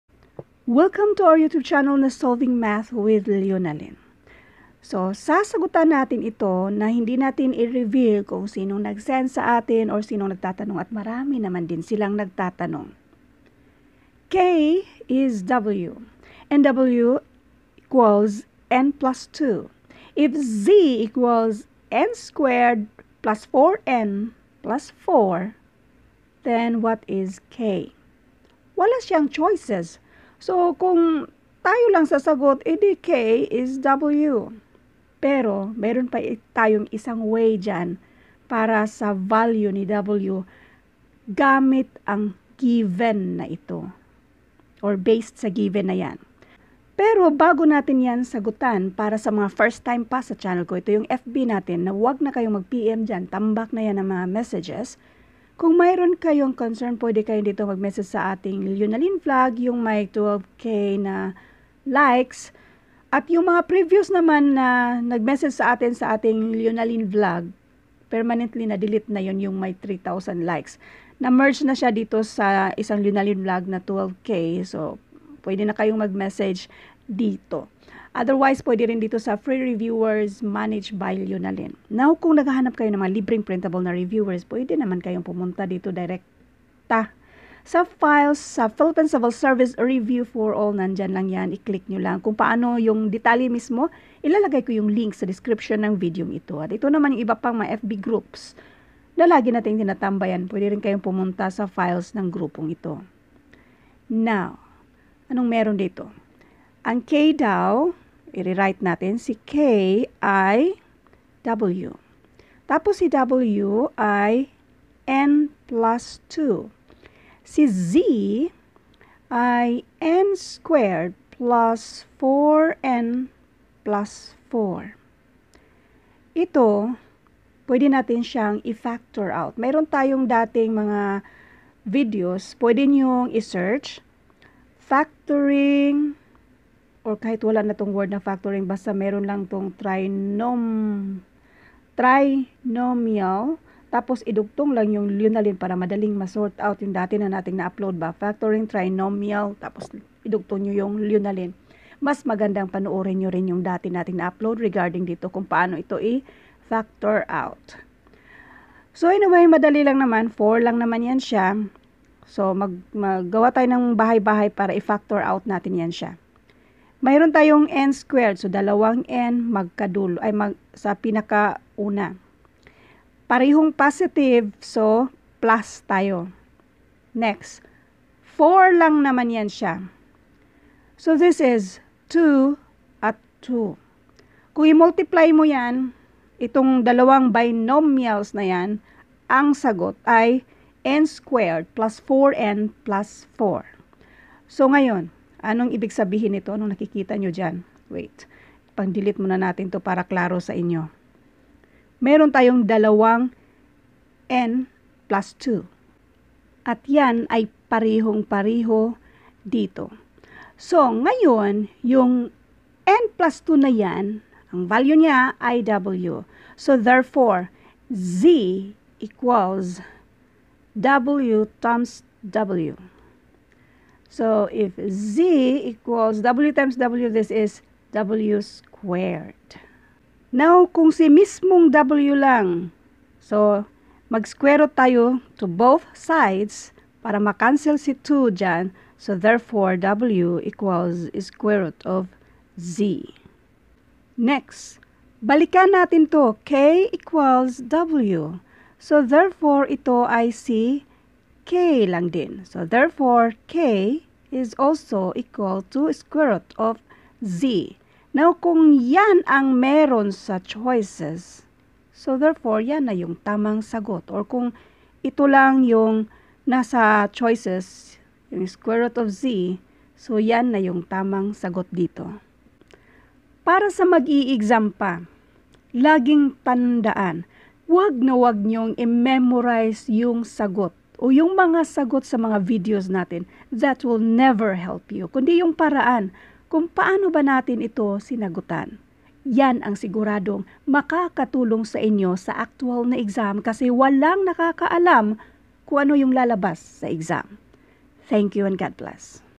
K is W, W = n+2, if Z = n² + 4n + 4, K is ? Civil Service Exam Numerical Reasoning
K is W, W = n+2, if Z = n² + 4n + 4, K is ?

Free Civil Service Reviewers managed by Leonalyn

Philippine Civil Service Review for all

Editing Apps: VivaVideo
SamsungNote5: Snote
Back-up: SamsungNote9